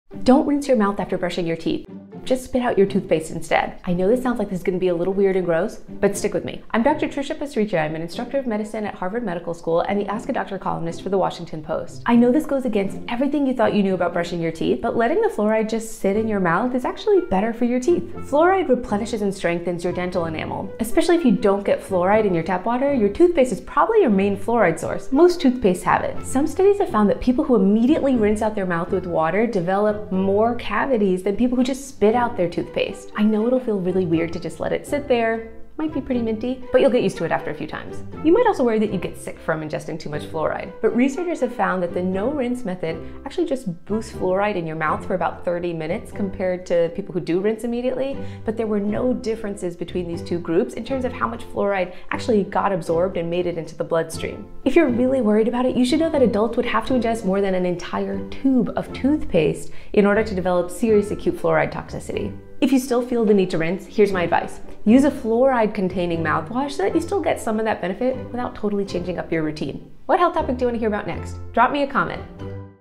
Don’t rinse your teeth after brushing. Here’s why.
Fluoride strengthens dental enamel, and toothpaste is probably your main fluoride source, especially if you don’t get fluoride in your tap water. Internal medicine physician, Harvard Medical School instructor and Ask a Doctor columnist Trisha Pasricha explains the benefits of fluoride.